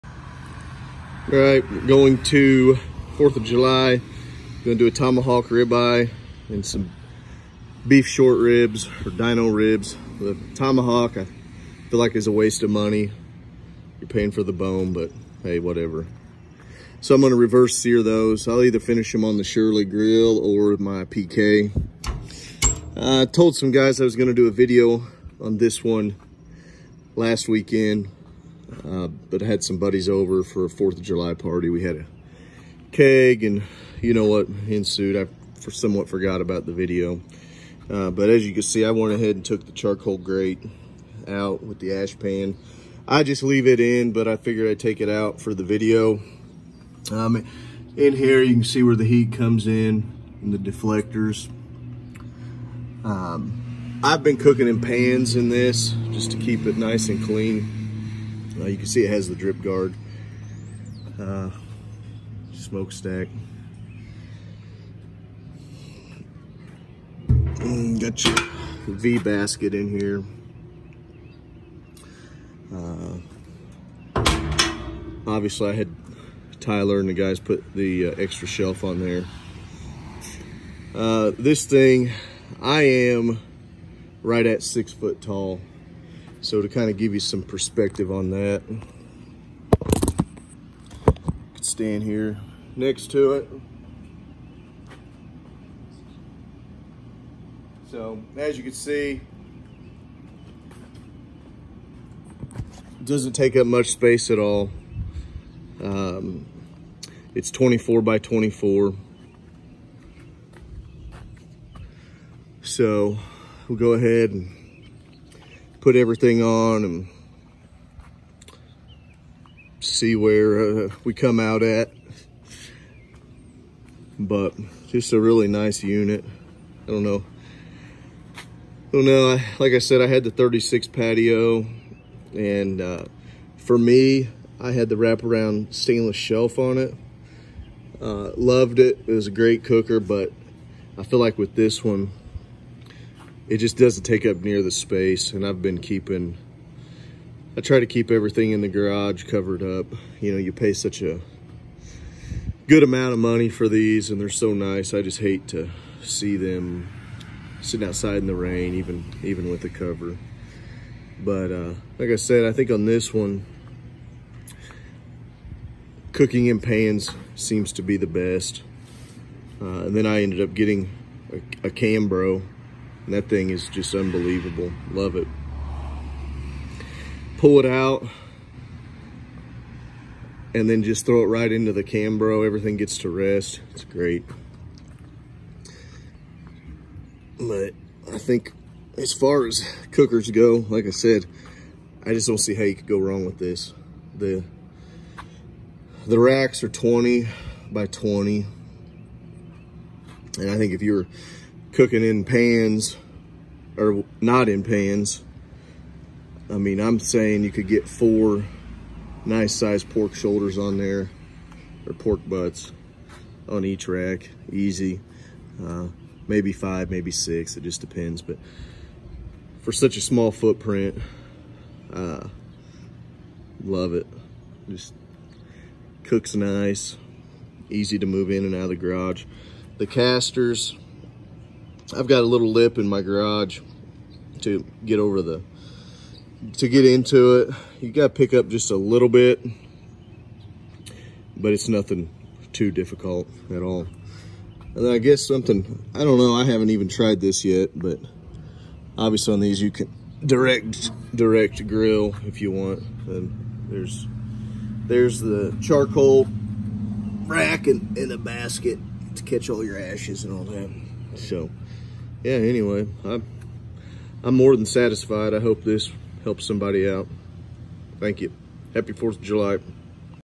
Shirley Fabrication Vertical Cabinet Walk Around
Walk around video of the Shirley vertical cabinet model